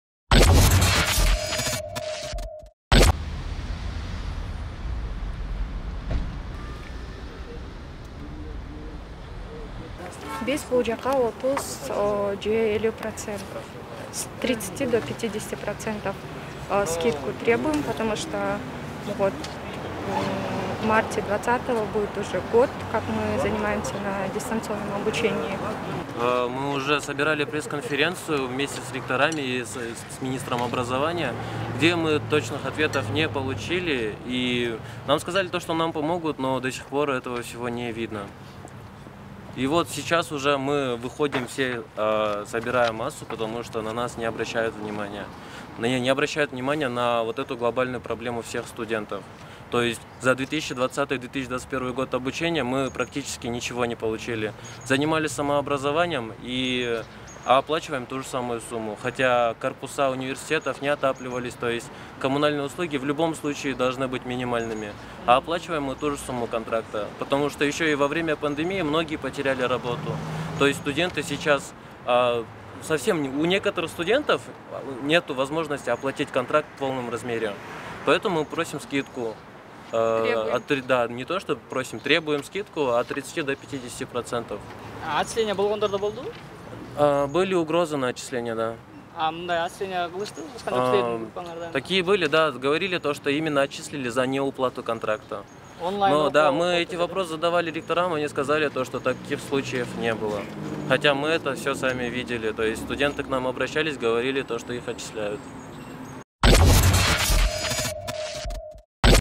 Окуу акысын төмөндөтүү. Студенттер митингге чыгышты
Бүгүн, 15-февралда Билим берүү жана илим министрлигинин алдында студенттер митингге чыгышты. Нааразычылык акциясына чыккандар окуу акысын 30 пайыздан 50 пайызга чейин төмөндөтүүнү талап кылышууда.

“Пандемия башталгандан бери бир жыл бою үйдө онлайн окудук. Интернет, коммуналдык кызматтардын акысын өз чөнтөгүбүздөн төлөдүк. Университеттин электр энергиясын, жылуулугун жана башка чыгымдарын кетирген жокпуз. Буга чейин биз билим берүү министри, ректорлор менен биргеликте пресс-конференция өткөргөнбүз. Жыйынтыгында бизге жардам берилет деген, бирок жооп дагы деле жок”, – деди министрликтин алдына чыккан студент.

Белгилей кетсек, көпчүлүк студенттер окуу акысын төмөндөтүү талабын коюп келишет. Бирок Билим берүү жана илим министрлиги окуу процесси аралыктан жүргөнү менен ЖОЖдордо чыгымдар азайбаганын жүйө келтирип, жеңилдик берүүдөн баш тартууда.